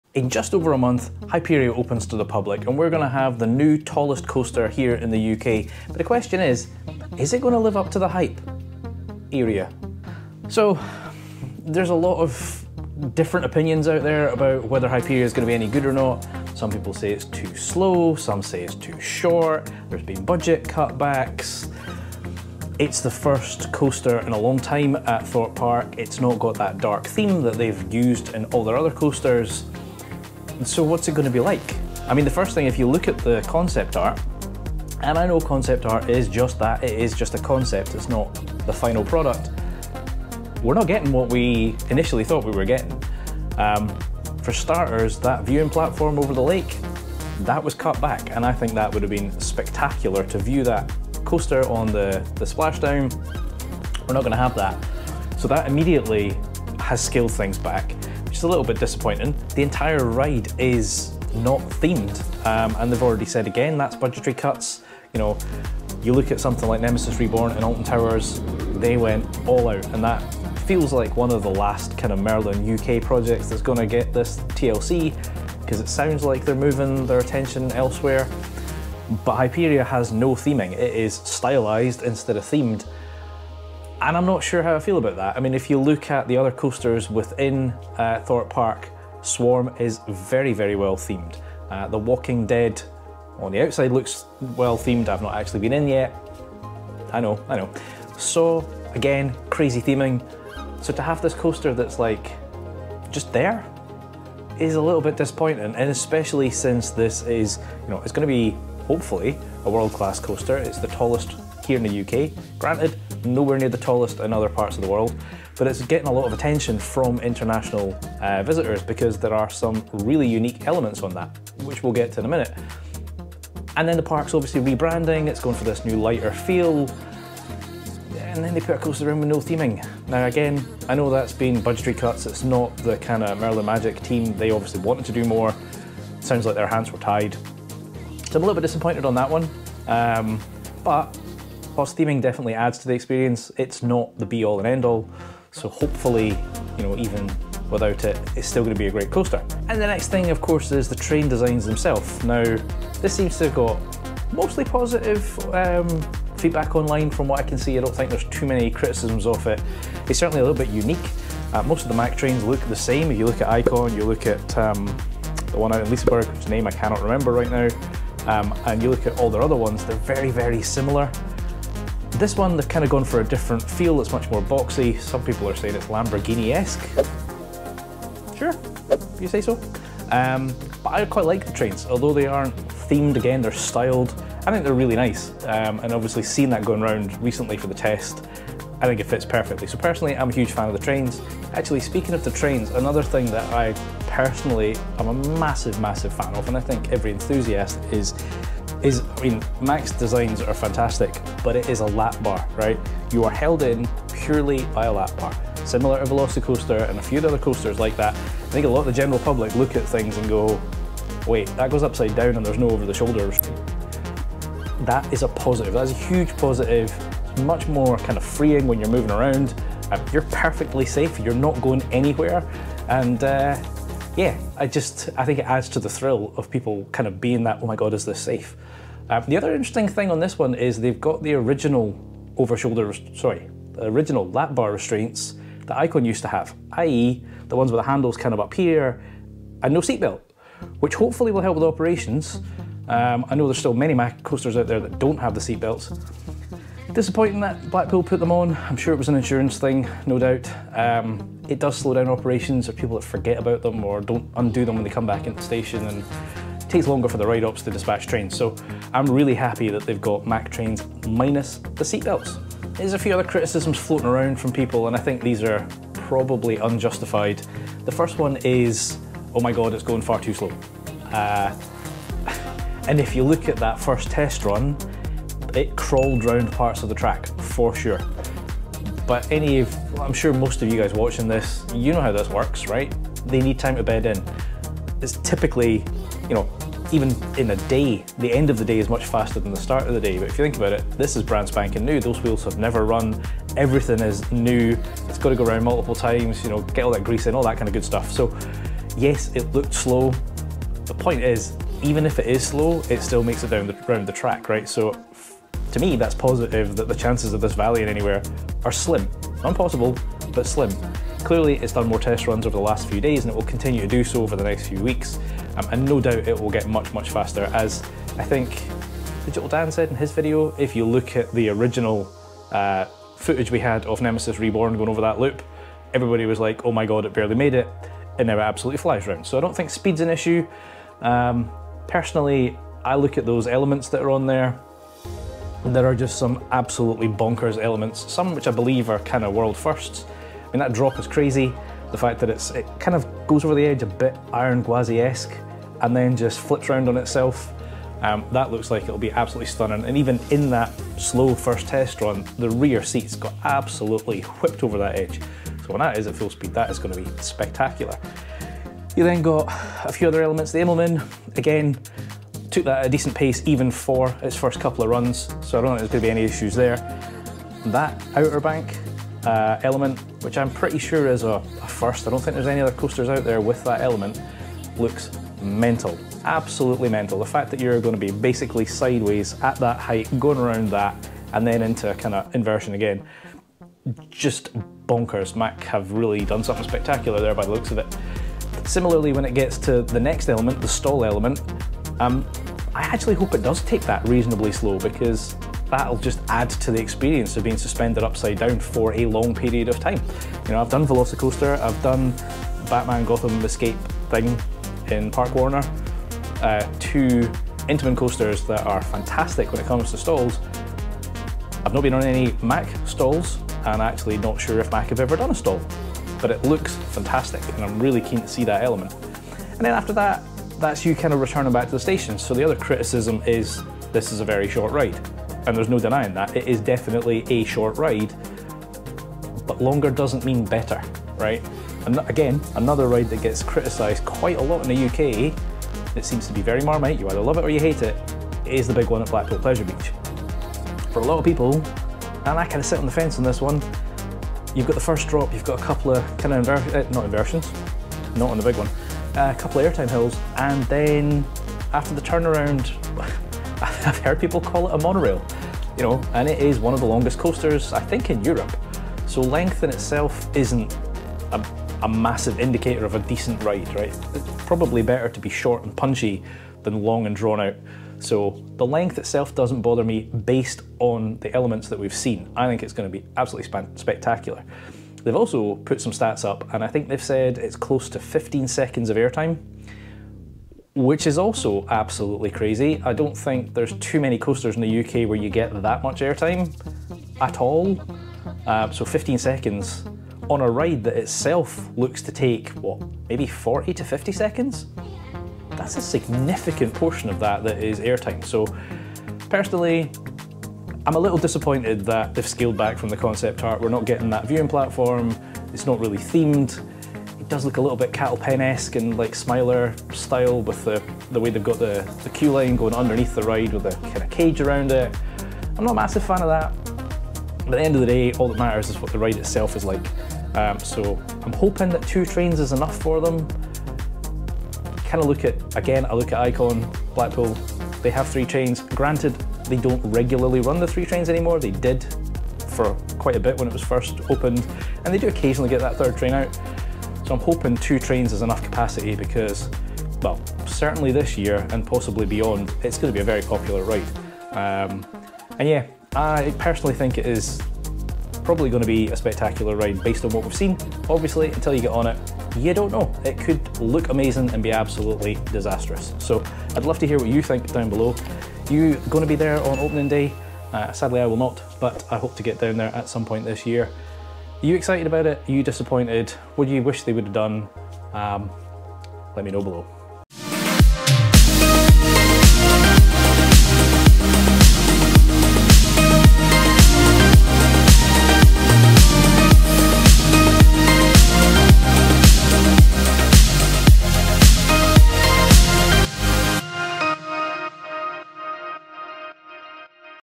Will it live up to the Hype-ria?
Join us as we explore Hyperia at Thorpe Park! Will this new attraction live up to the hype, or will budget cuts and lack of theming disappoint? Let's find out!

Follow Coaster Dad on social media!

If you like Theme Parks and puzzles, check out my books on Amazon: